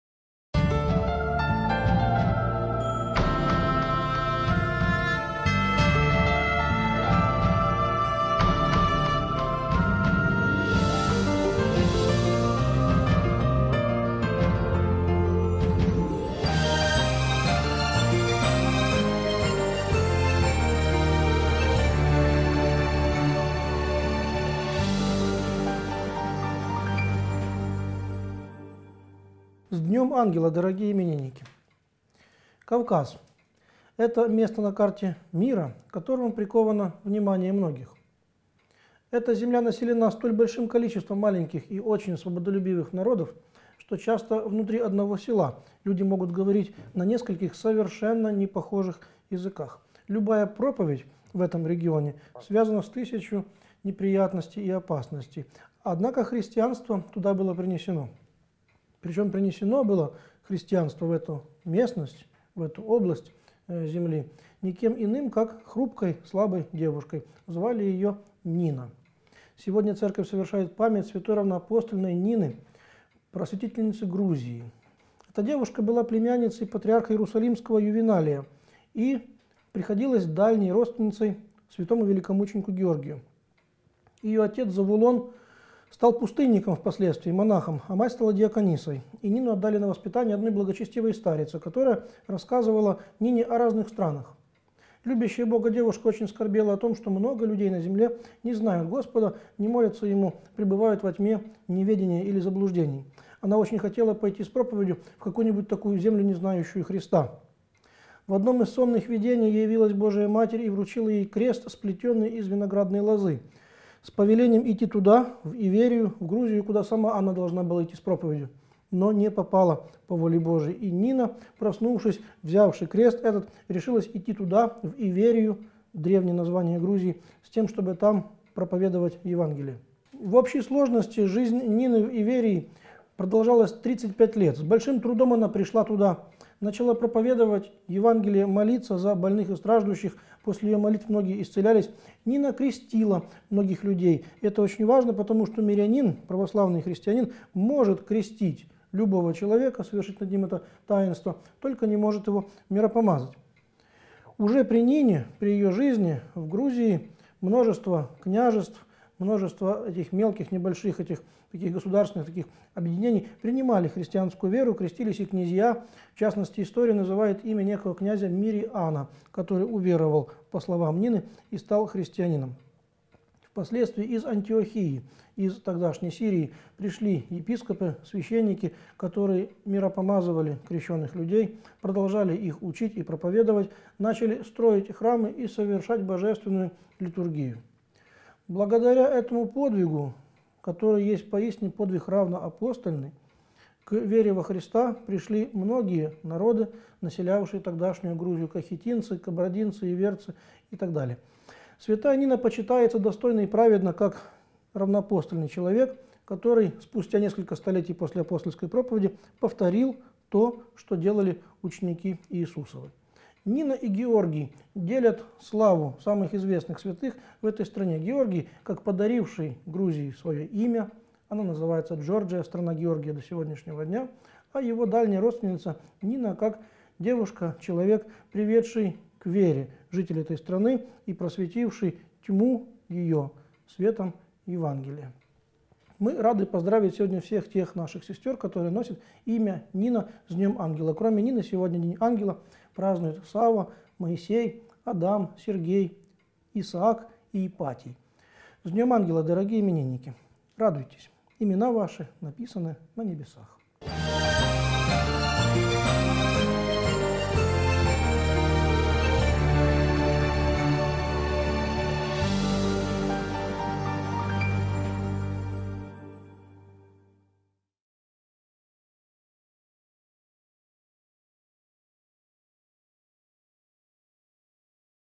День ангела 27 января – День равноапостольной Нины, просветительницы Грузии – отец Андрей Ткачёв
Распространи это видео!